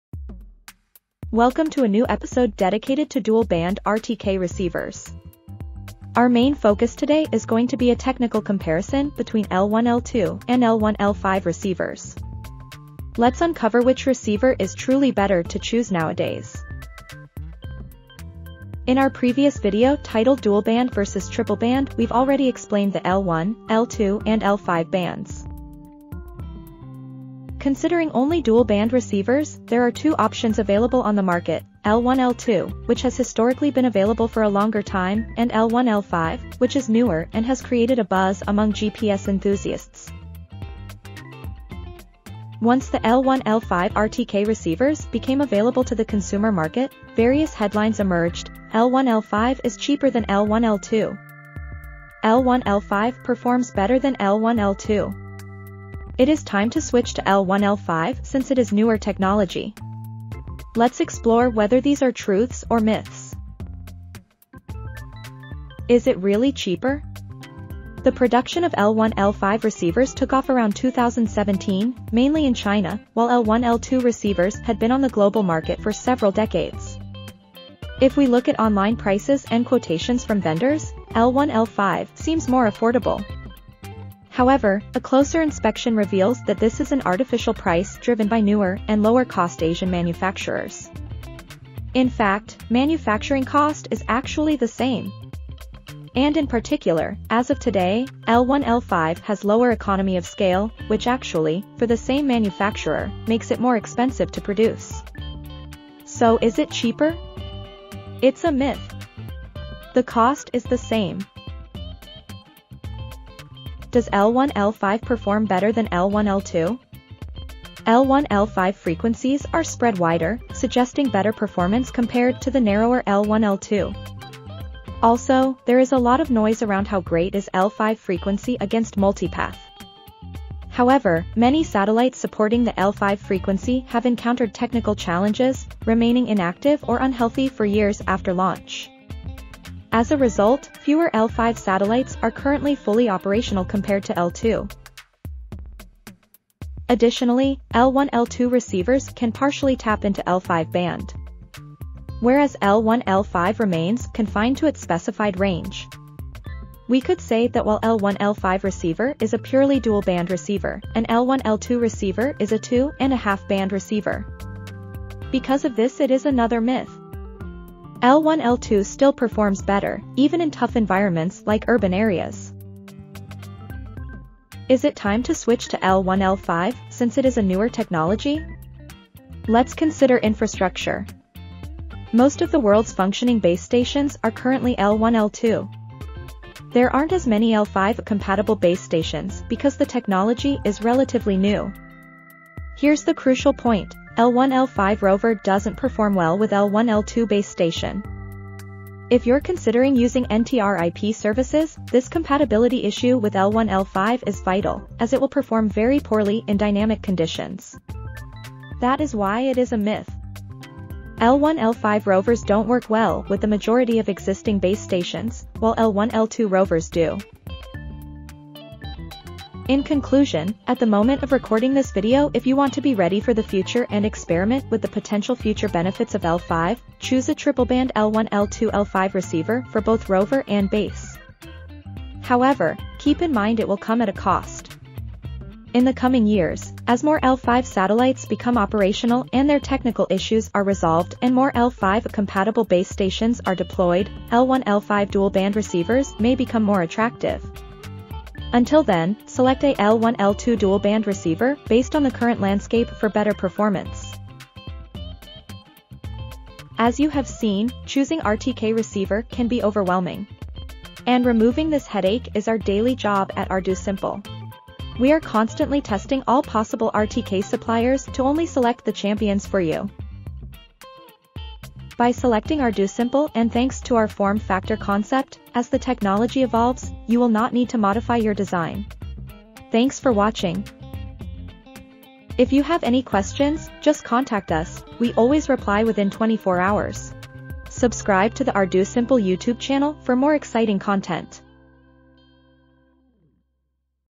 L1L2 vs L1L5: Which dual-band GNSS RTK receiver is better today?
Have a closer look at dual-band RTK GPS receivers. We're comparing two types – L1/L2 and L1/L5 – to see which one is better nowadays.
If you're interested in GNSS and want to know more about these receivers,  we tell you what's true and what's not!

🌟 What We Cover:
0:40 Myth-Busting: With a lot of information floating around, we separate fact from fiction about these receivers' cost-effectiveness and performance.
1:05 Is L1+L5 cheaper than L1+L2? Delve into the specifics of receivers, discussing their history, pricing, and more.
1:53 Does L1+L5 perform better than L1+L2?  Learn about the frequency ranges, satellite availability etc that affect on the performance.
2:54 Is it time to switch to L1+L5 since it is newer technology?  Let’s consider infrastructure. Most of the world's functioning RTK Base stations are currently L1L2. Are they compatible with newer L1L5 rovers?
3:39 Future-Proof Choices: Discover how choosing the right receiver today can impact your projects
4:16 Our Role at ArduSimple: We're here to make choosing the right RTK receiver easy for you. We test and choose the best ones so you don't have to worry.

📣 Are you new to RTK and don't know what products fit your needs?